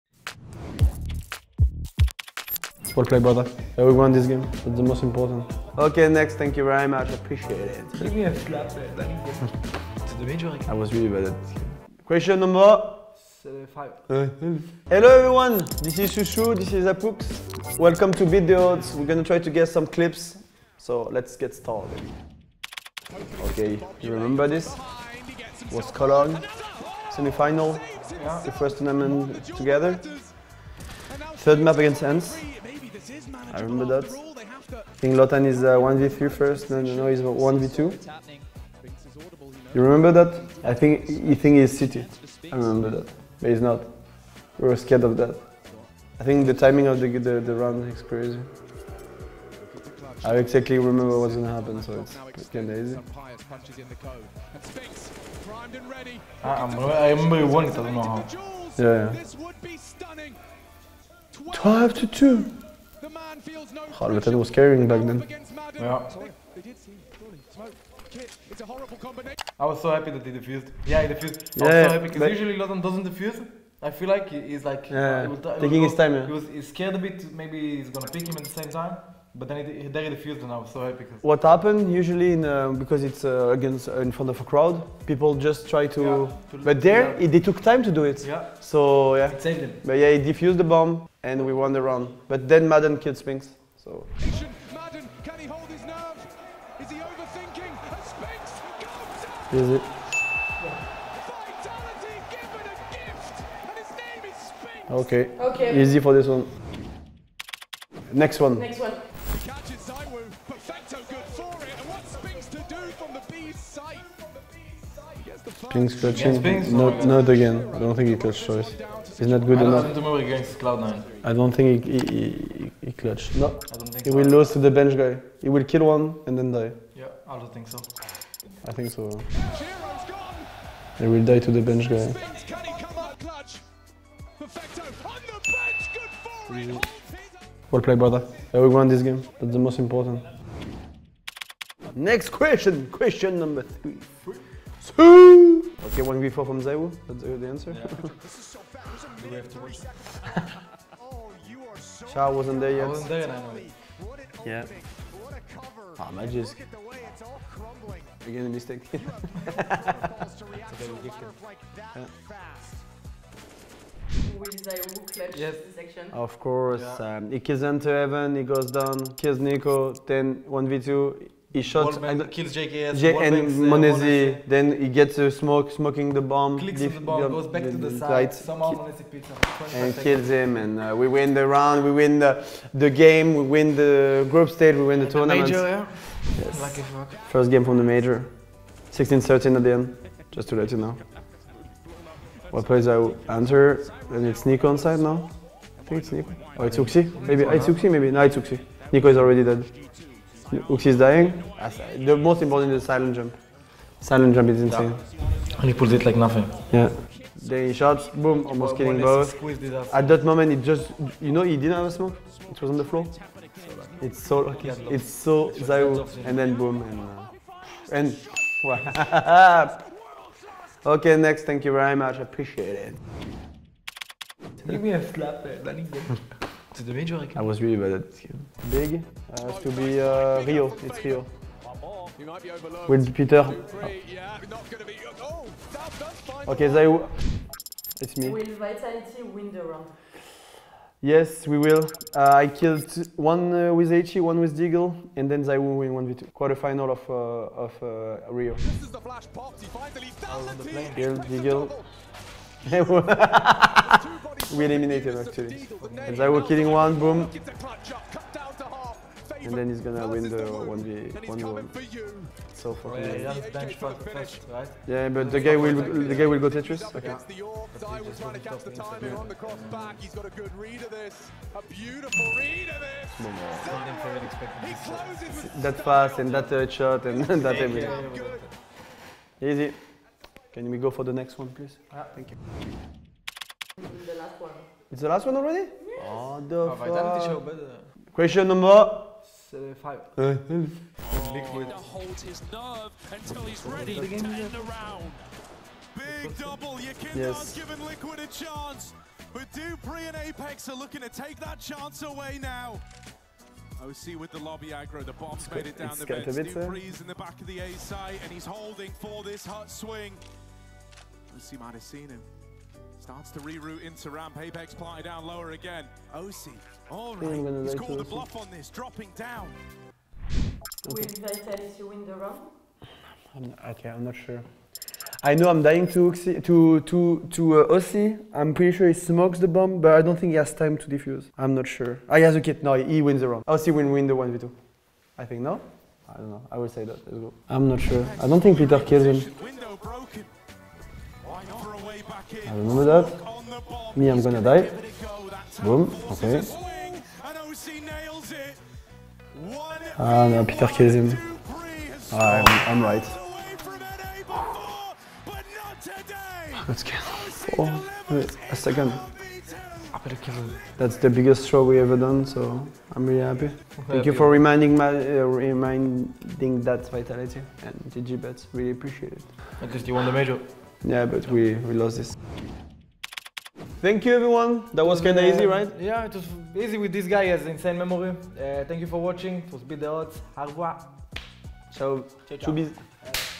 How good are apEX and flameZ in predictions? — Find out with in Beat the Odds by GG.BET
Team Vitality has proven countless times their ability to beat the odds as players. Now, GG.BET decided to see how well they fare in predictions!
In the debut episode of Beat the Odds we invited apEX and flameZ. Tune in to see how they tackled the challenge!